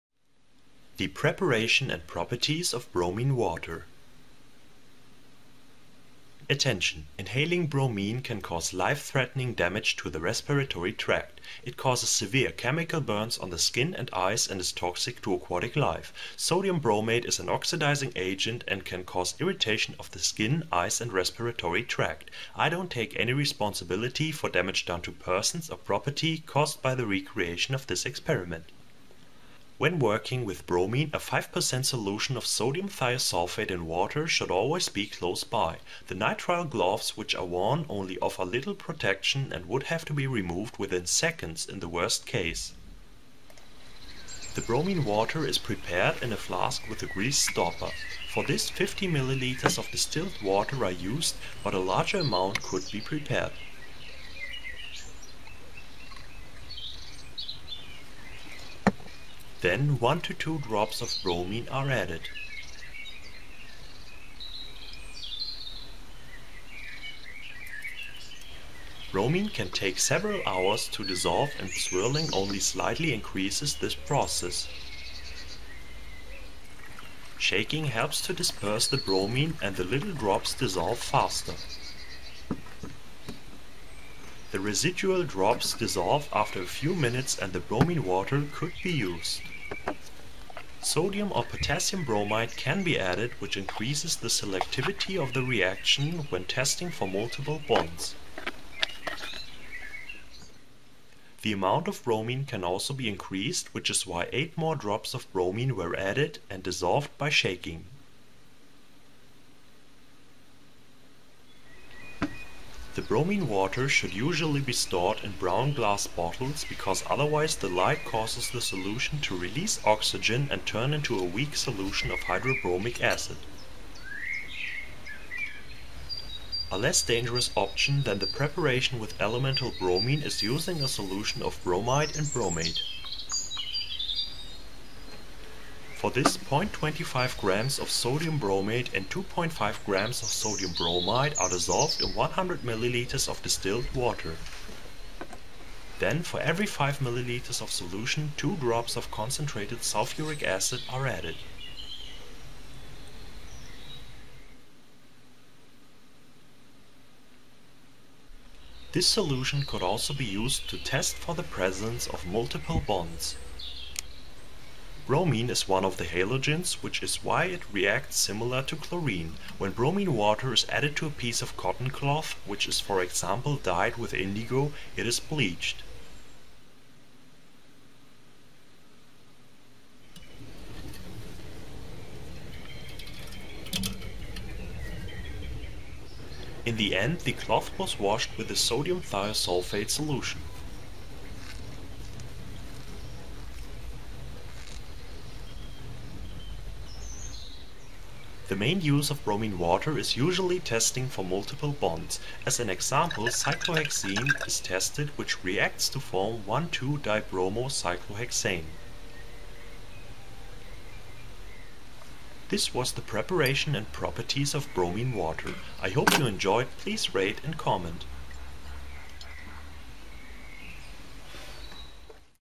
Bromine part 2 - Preparation and properties of bromine water
In this video two methods are shown to prepare bromine water. And some of it properties are demonstrated.